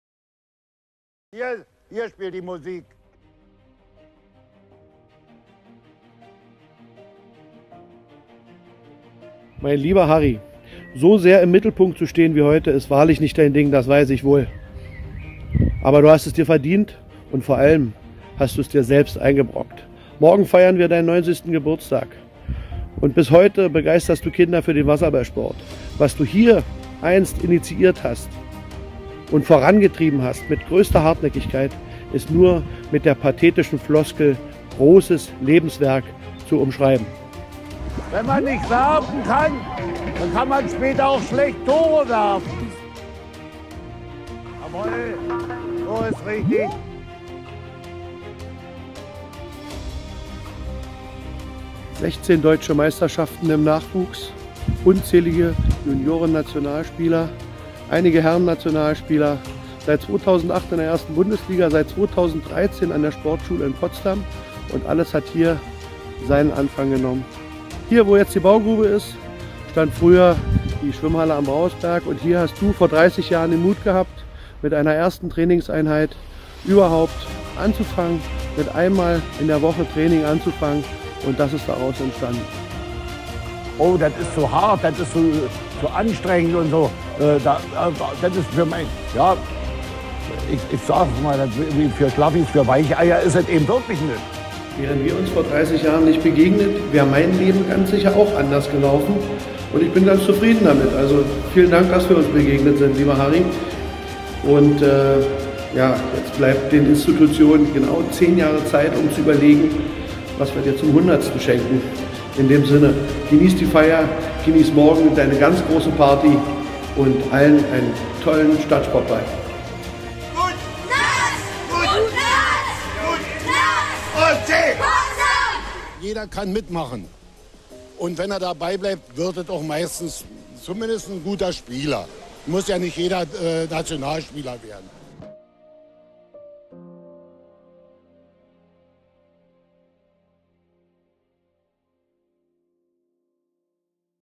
2022 (14.05.), Ehrung Harry Volbert auf dem 23. Stadtsportball Potsdam
Nach der Laudatio von Ute Goldberg, der Vorsitzenden des SSB-Potsdam, wurde der Film auf der großen Leinwand der MBS-Arena abgespielt.  Harry Volbert erhielt an diesem Tage einen Pokal für sein sportliches Lebenswerk, vom Landessportbund Brandenburg die Ehrenplakette und durfte sich, nach einer bewegenden Rede von Oberbürgermeister Mike Schubert, ins goldene Buch der Stadt Potsdam eintragen.  Harry Volbert belebte 30 Jahre zuvor den Wasserballsport in Potsdam wieder.  Der Sportball fand einen Tag vor seinem 90. Geburtstag statt.